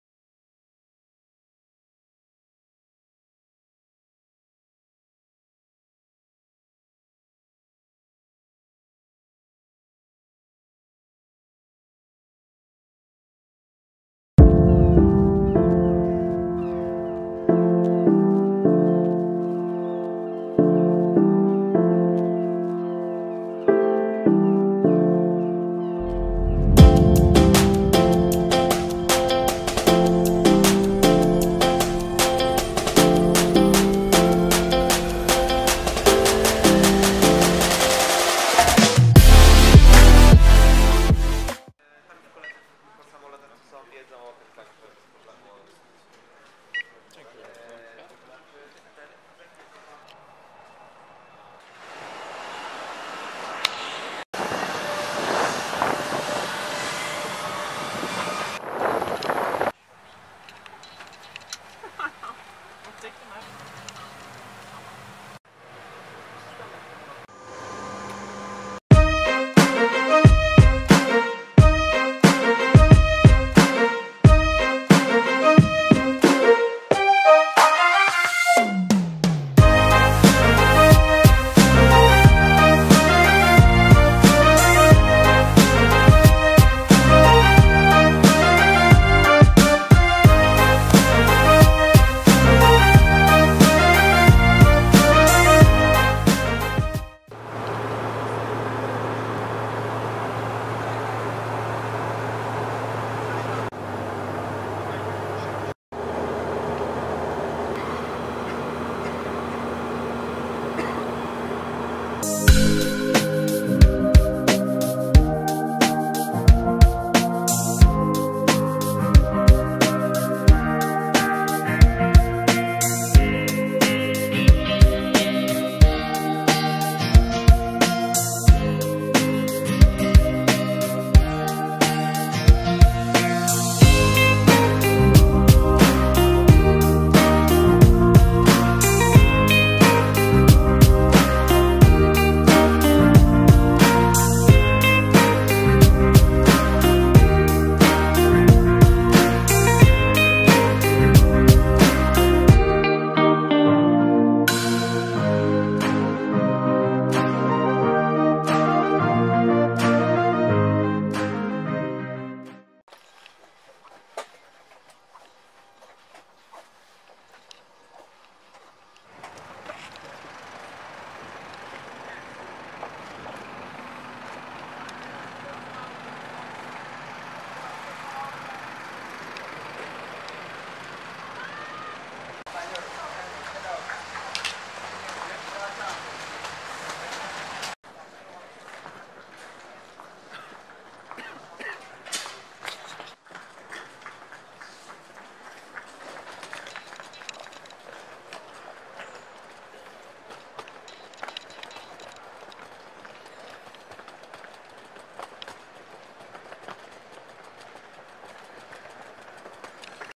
Flight Review | CRJ 900 Adria Airways | Warsaw to Vienna
Report from my quick visit in Vienna. This time I had a opportunity to fly with Austrian Airlines. The first leg to Vienna was operated by Adria Airways, Bombardier CRJ900.

Flight details:
- Flight number: OS624
- Aircraft reg: S5-AAL
- Seat number: 22A

Made by:
- Nikon D5300